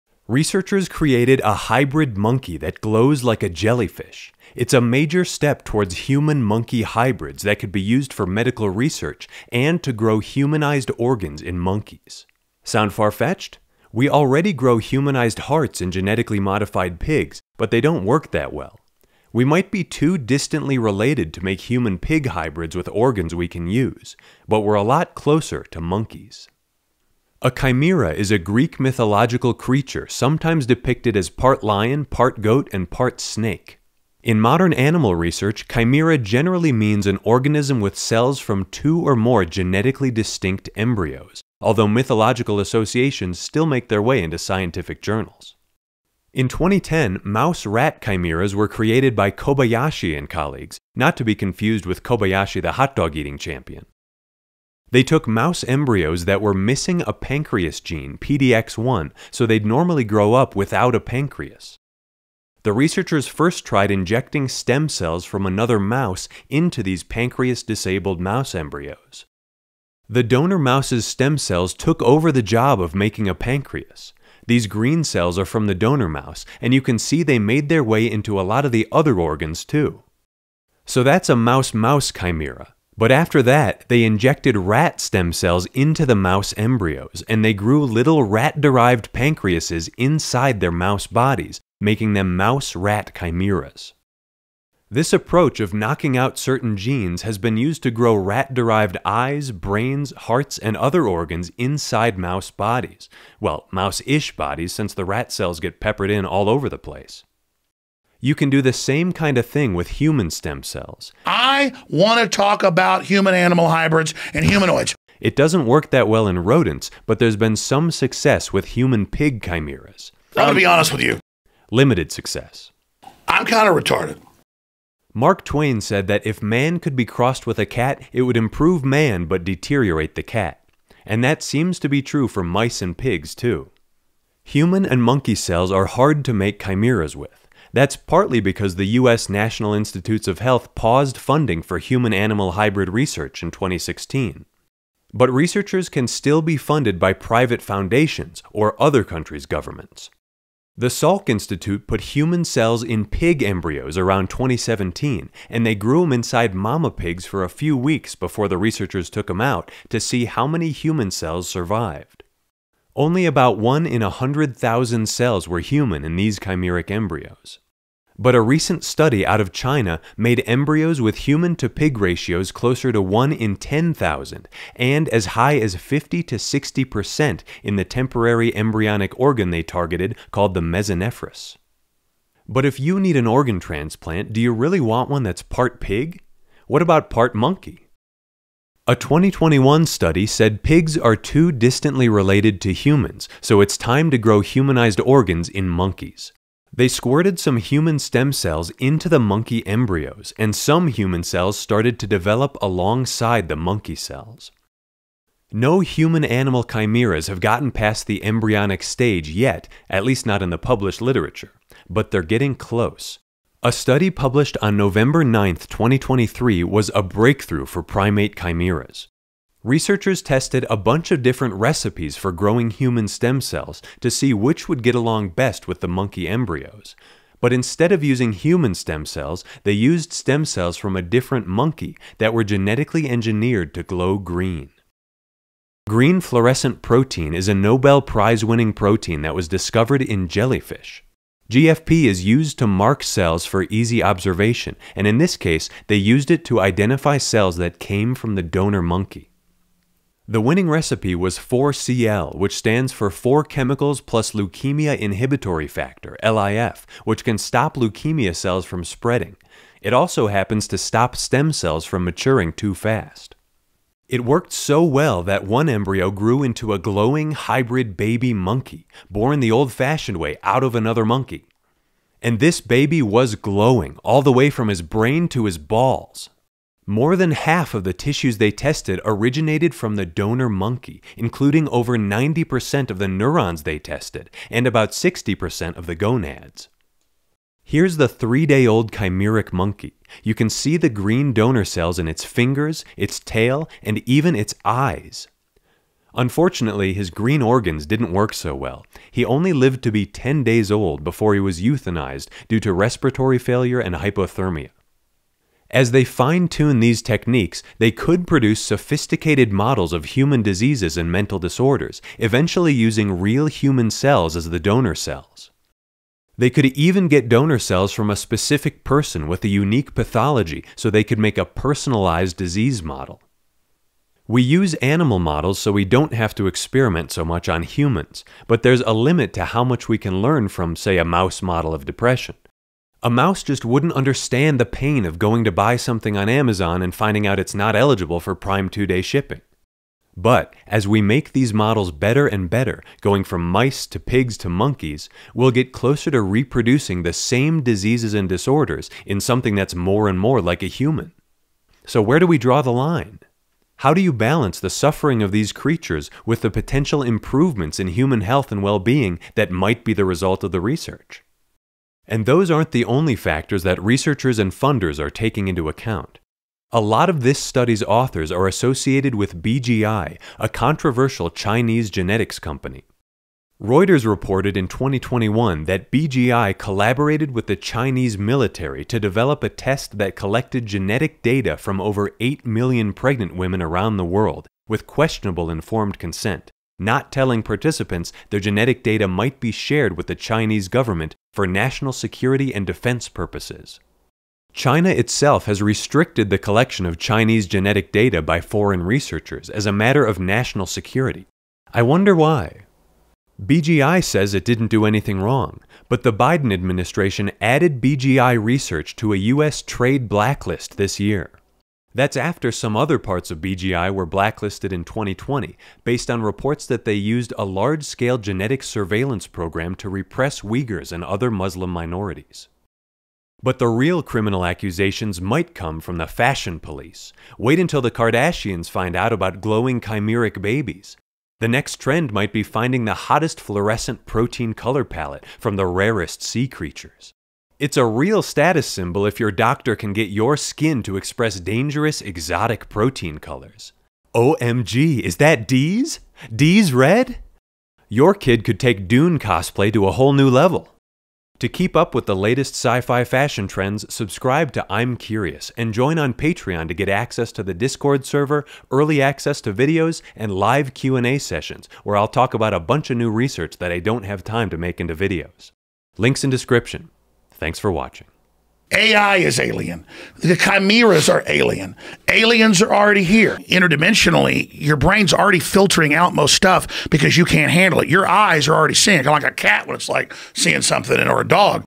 Hybrid Monkey Breakthrough: A Step Toward Human-Animal Chimeras
Researchers created a hybrid monkey that glows like a jellyfish. It's a major step toward human-monkey chimeras and humanized organs grown in monkeys.